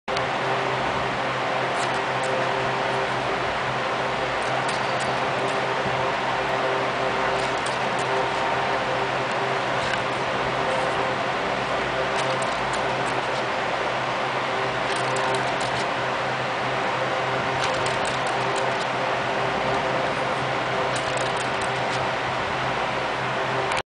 Drum Brake Demonstration
Drum Brake model made from cardboard.